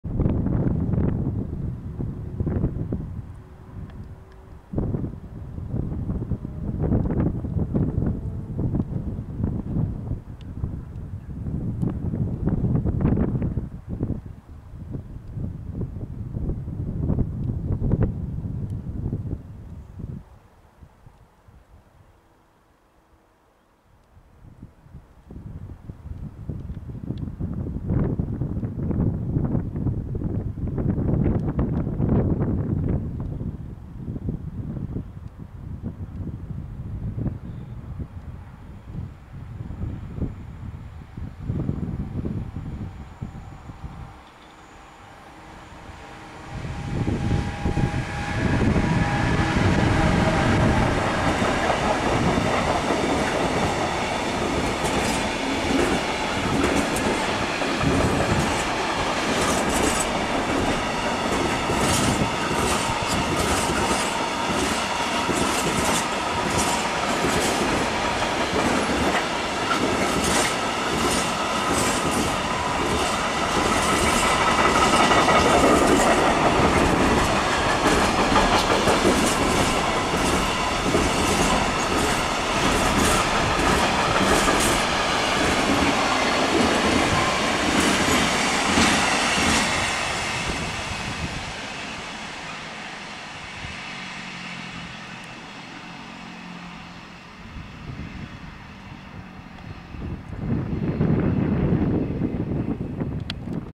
193 791
Vectron RTB Cargo ze składem ze Starej Wsi do Niemiec niespiesznie przejeżdża przez Konin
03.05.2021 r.